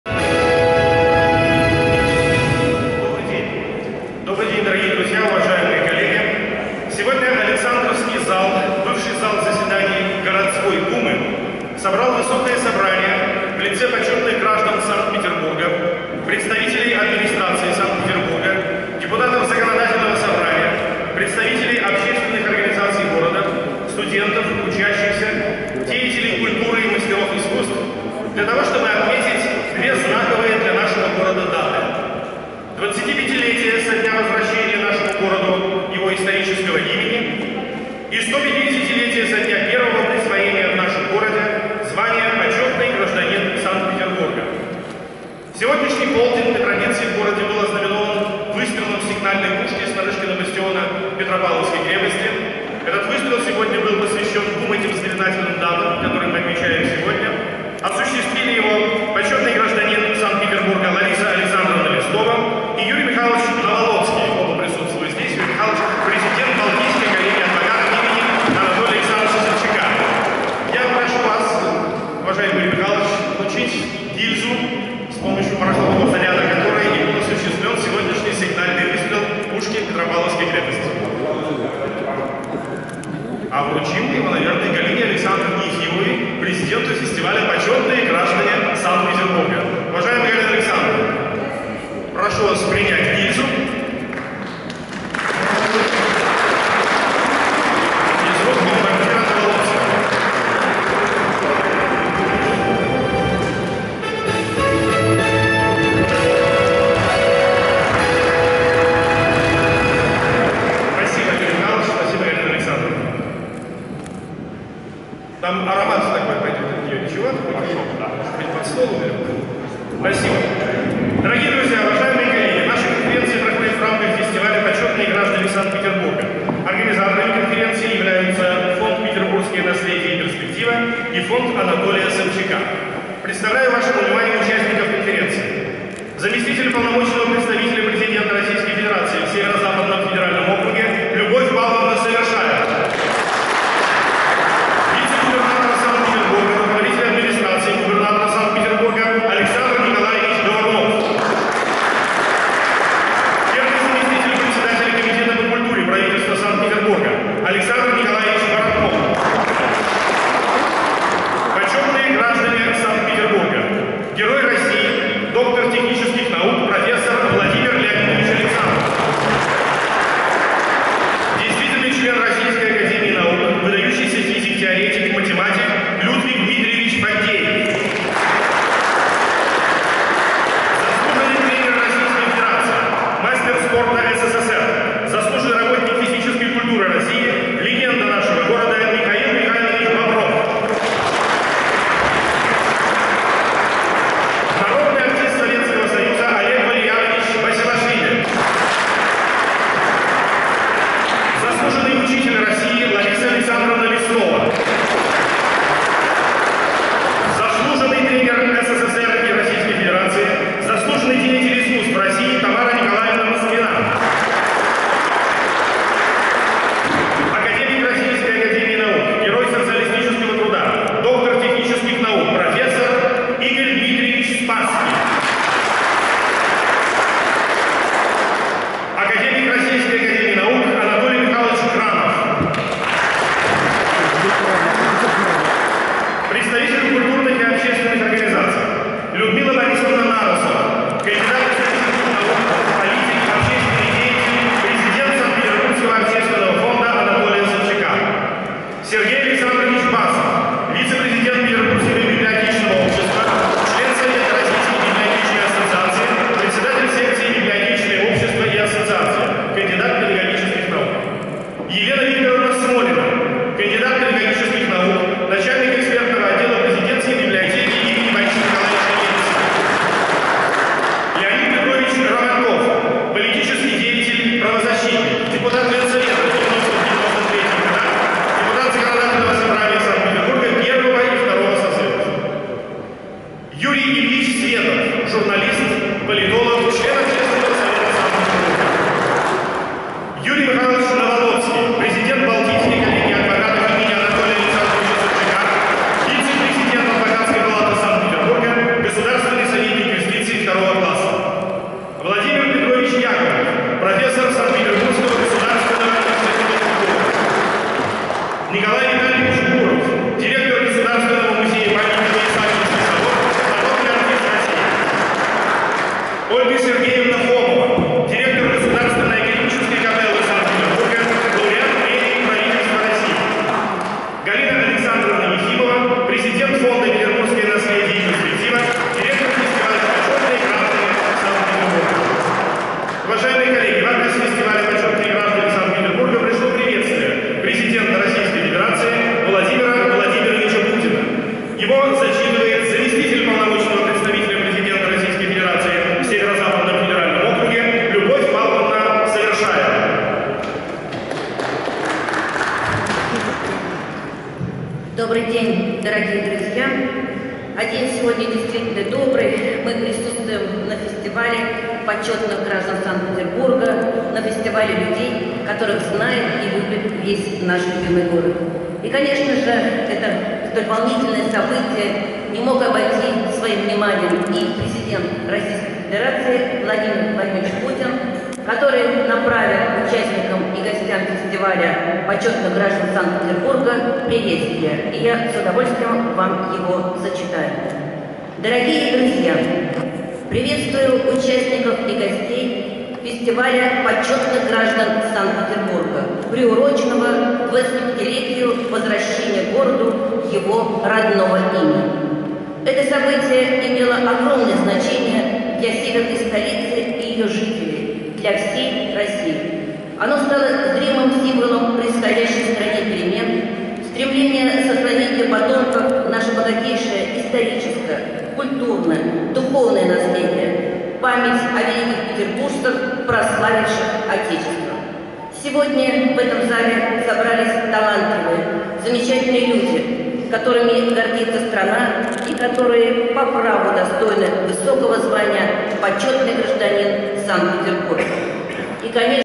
Открытие конференции
9 апреля в Александровском зале Городской Думы конференция. посвященная 150-летию звания "Почётный гражданин Санкт-Петербурга" и 25-летию возвращения исторического названия нашему городу